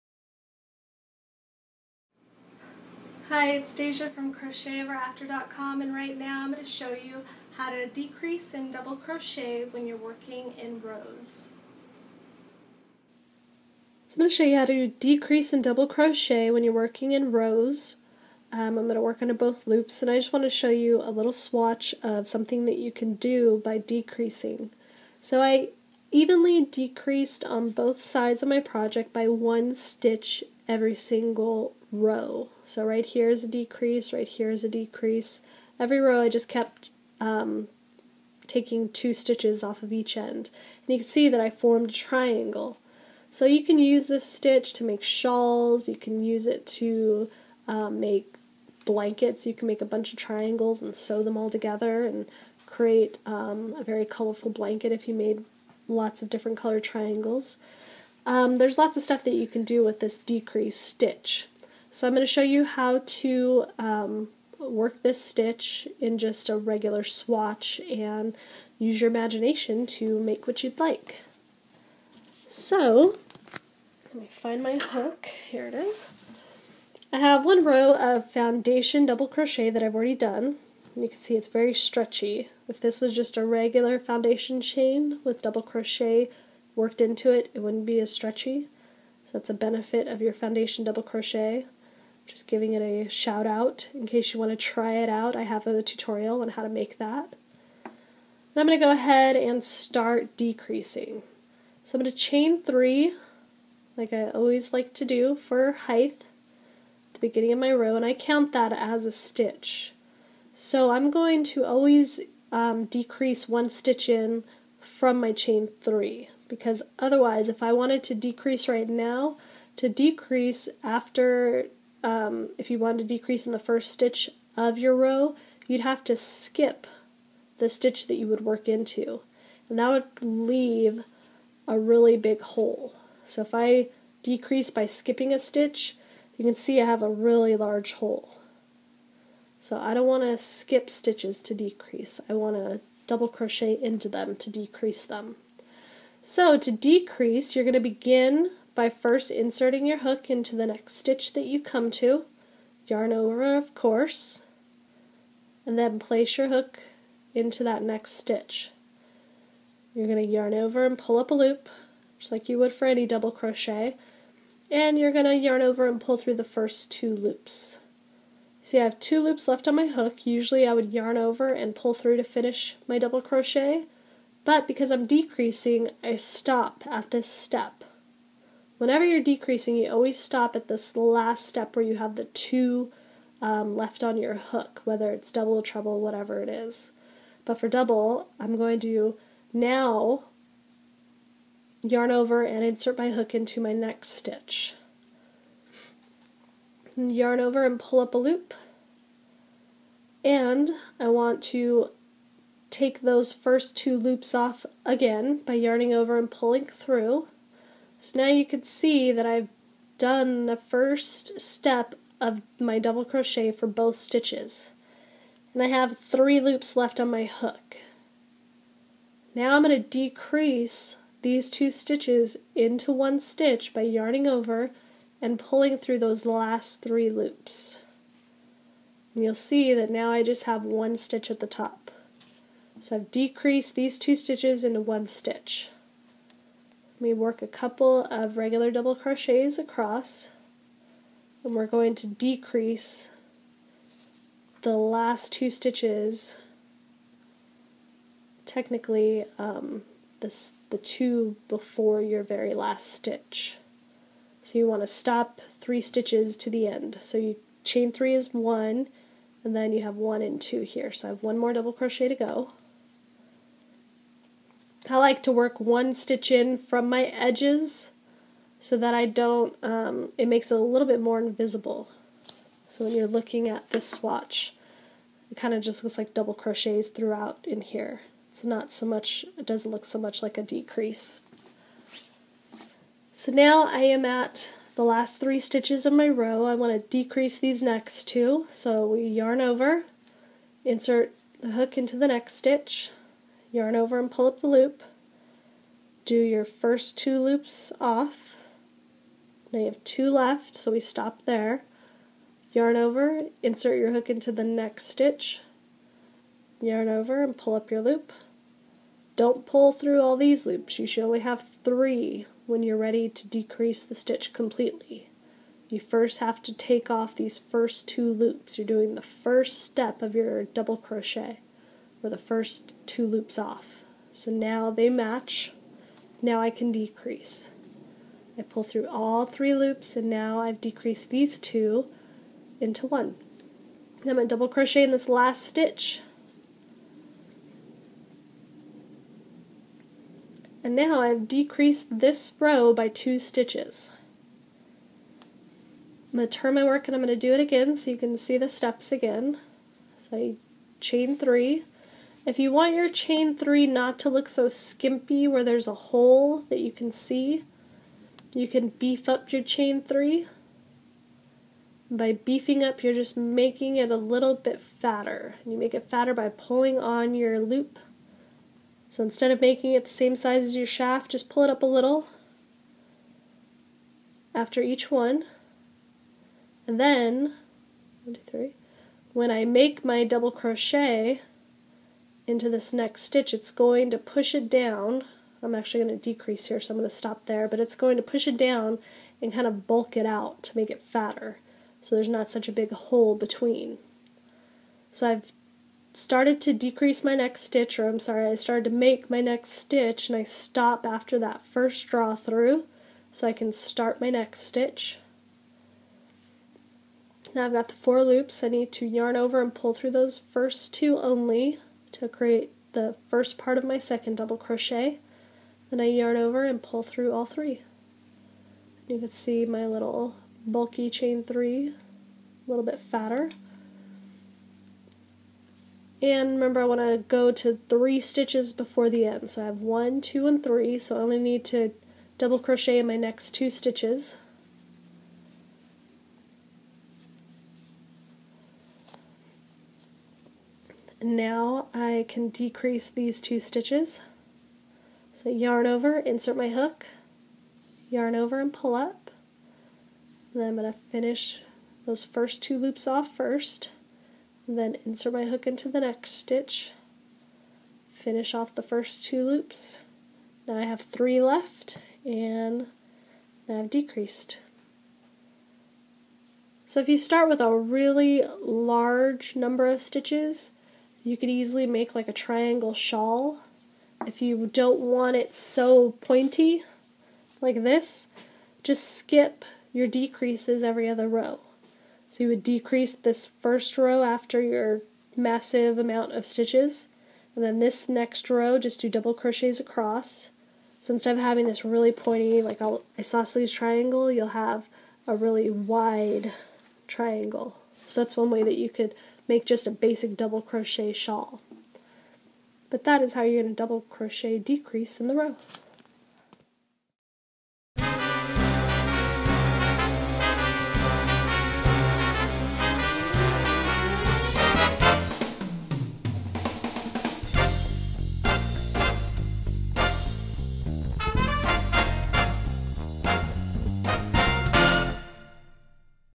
Left Hand Double Crochet Tutorial #6: DC Decreasing (DC2TOG)When Working in Rows
See the left hand perspective of decreasing in double crochet when working in rows.